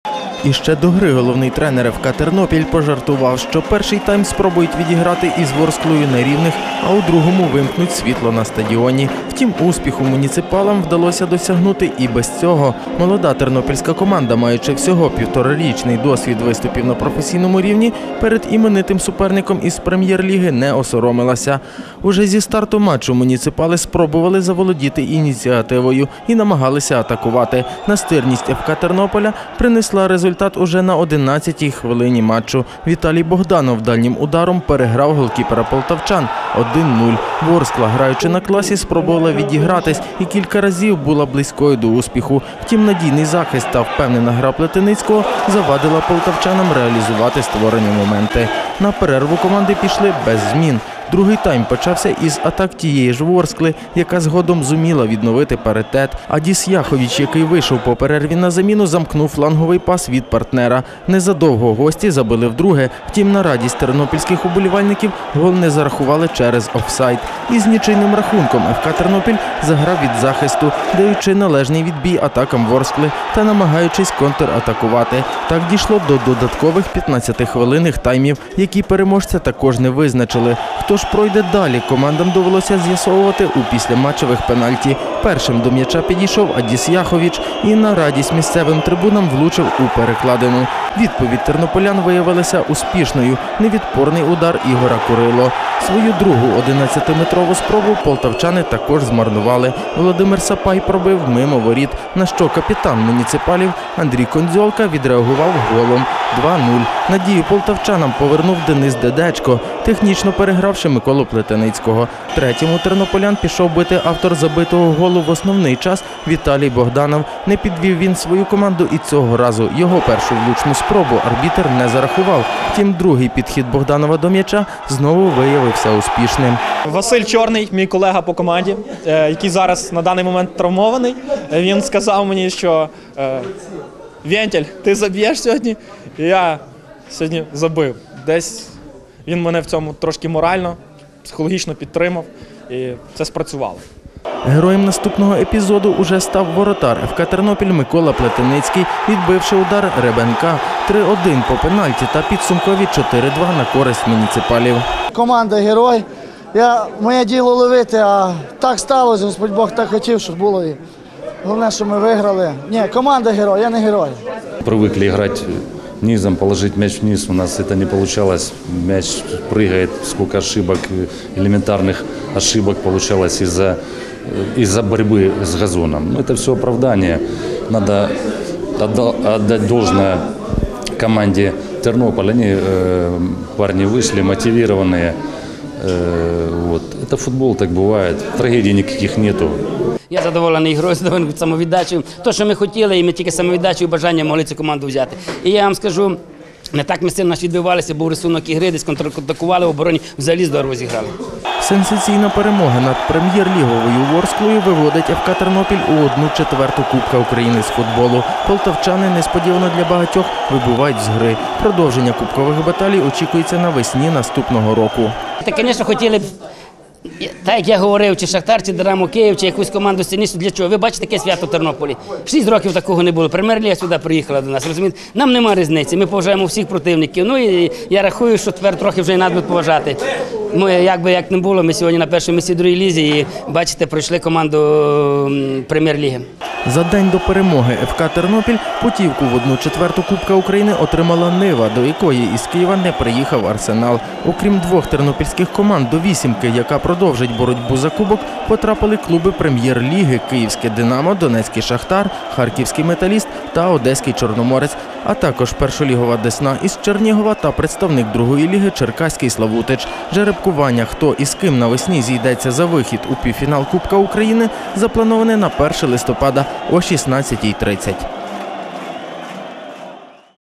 Як ФК "Тернопіль" перемагав "Ворсклу"
ФК "Тернопіль" сенсаційно перемагає "Ворсклу" і виходить в 1/4 Кубка України з футболу. На матч року, як його охрестили в Тернополі, вболівальники муніципальної команди заповнили стадіон чи не вщент, і попри те, що суперником був один із кращих клубів прем'єр-ліги, вони стали свідками не лише свята футболу, але й перемоги.